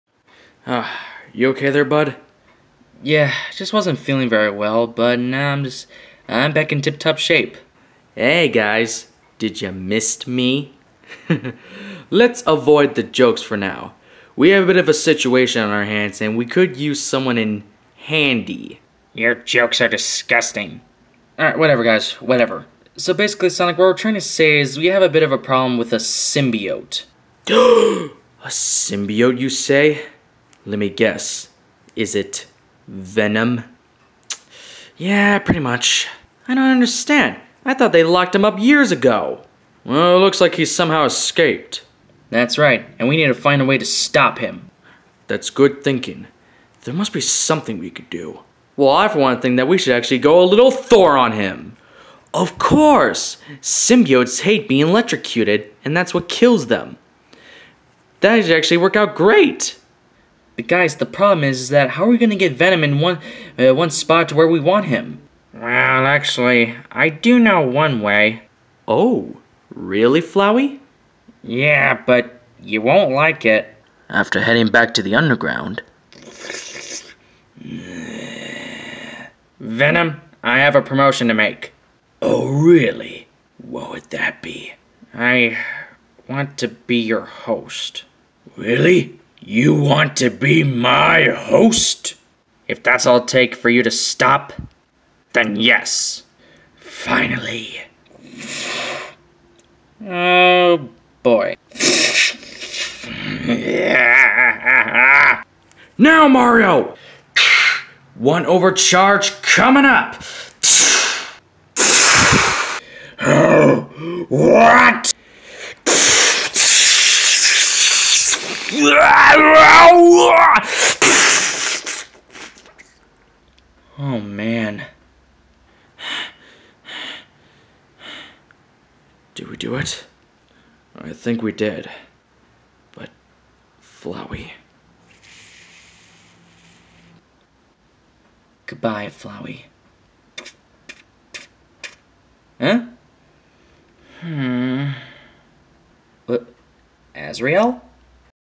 Tenzin returns: part 1 / episode 4: megalomaniac
Tenzin and the others have a final showdown with venom.